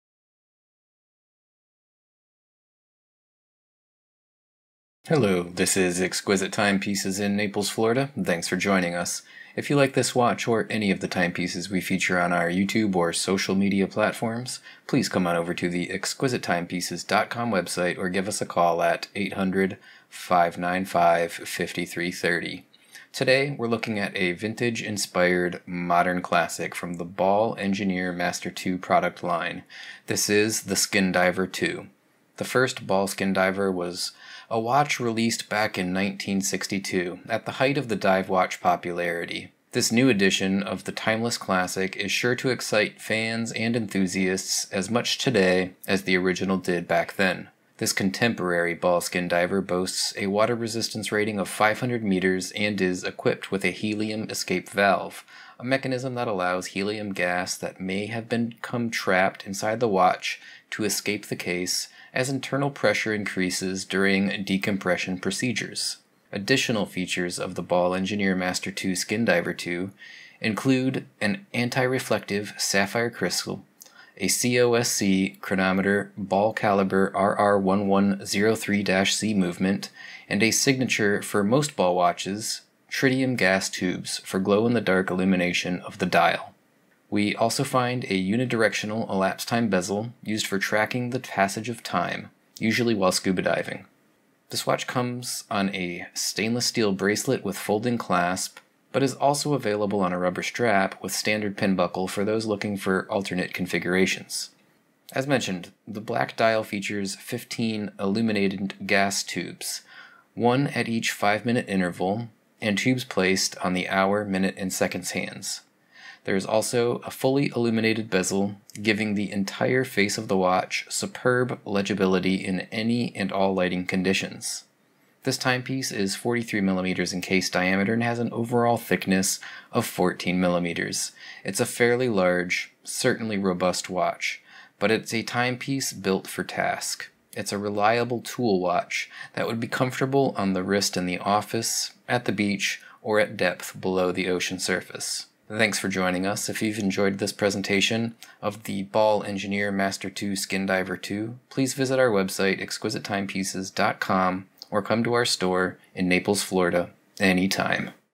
Ball Watch Engineer Master II Skindiver DM3108A-SCJ-BK Watch Review 2019
Ball Watch Engineer Master II Skindiver DM3108A-SCJ-BK

A new vintage diver's watch that pays tribute to BALL's watchmaking heritage

In 2012, BALL Watch Company took up the bold challenge of reviving a resonant part of its watchmaking heritage in the form of the Engineer Master II Skindiver. Following the unanimous success of this model as soon as it was launched, BALL Watch Company now once again meets the expectations of all lovers of vintage diver's watches with the new Engineer Master II Skindiver II.

Just like its predecessor, the Engineer Master II Skindiver II takes its inspiration directly from the first BALL Skindiver" timepiece conceived in 1962 in response to the increasing popularity of underwater diving in the United States, the original homeland of BALL watches. The launch of this watch followed the creation of the "Underwater Society of America" in 1959. This public organization is responsible for promotional work and providing training in relation to diving, as well as organizing competitions. A practice dating back several thousand years, diving developed as a sport and method for exploring the seabed after the Second World War. It gradually split into two codes: scuba diving and skin diving or free diving.

For many years now, BALL Watch has played a pioneering role in the design of diver's watches, especially through its extraordinary partnership with the multiple world champion constant weight freediver Guillaume Néry. This outstanding human and technical exploit has been a source of inspiration for an entire range of diver's watches at the cutting edge of modern watchmaking technology. Once again, BALL Watch is proud to introduce a new model into the ranks of the Engineer Master II collection, paying tribute both to the history of the watch itself and to the heritage of underwater exploration."

* Reference: DM3108A-SCJ-BK
* Country of Manufacture: Switzerland
* Bracelet Strap: Stainless Steel
* Case: Stainless Steel
* Case Thickness: 14mm
* Case Width: 44mm
* Crystal: Sapphire Crystal
* Crown: Screw Down
* Dial Color: Black
* Functions: Hours, Minutes, Seconds, Date
* Gender: Men's
* Hands: Micro Gas Tubes
* Dial Markers: Micro Gas Tubes
* Brand: Ball
* Warranty: Full Factory Warranty
* Movement: Automatic caliber BALL RR1103-C
* Water Resistance: 500M